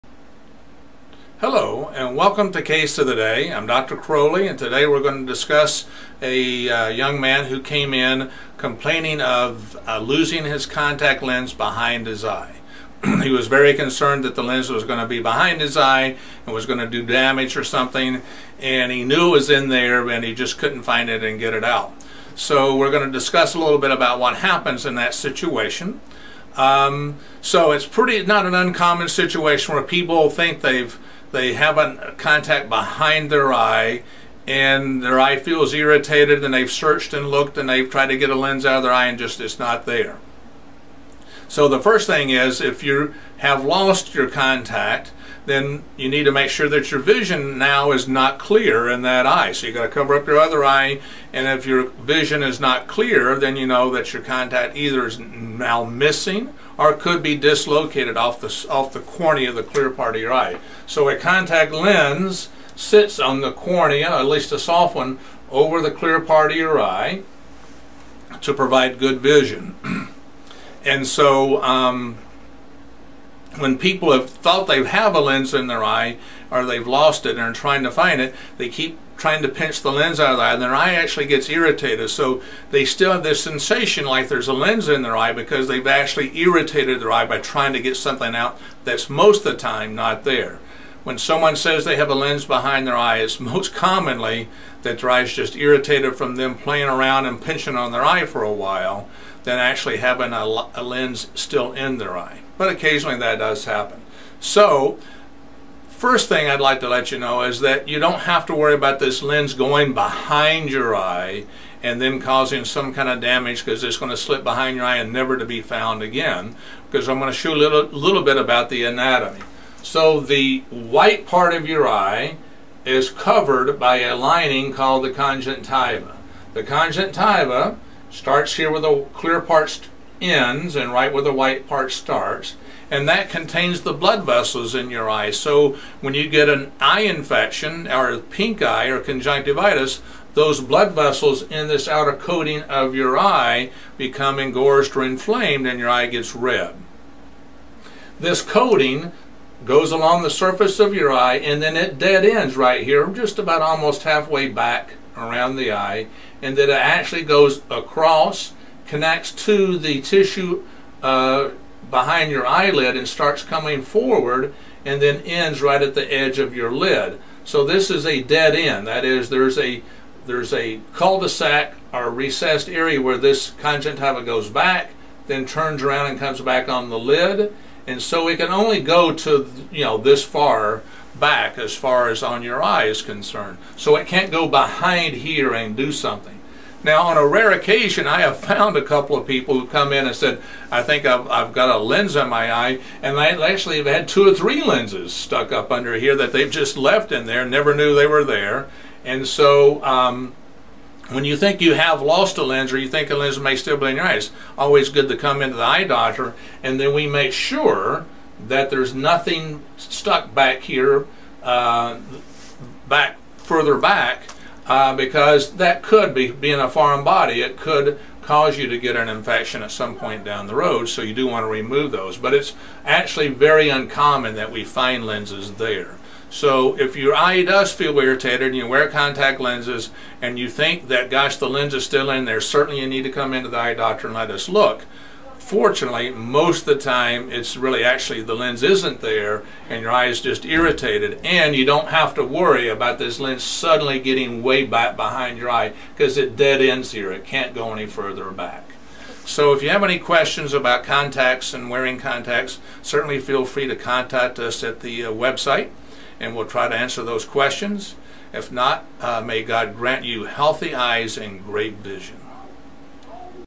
Case of the Day- Lost Contact Lens Behind the Eye
Dr. Croley describes a patient who thought their contact lens was stuck behind their eye. Many patients believe that a contact lens may slight off their cornea and slip behind the eye. The anatomy of the eye prevents the lens from going that far as the conjunctiva ends about half way around the eye posterior from the front of the eye. In most cases the contact lens has been lost from the eye.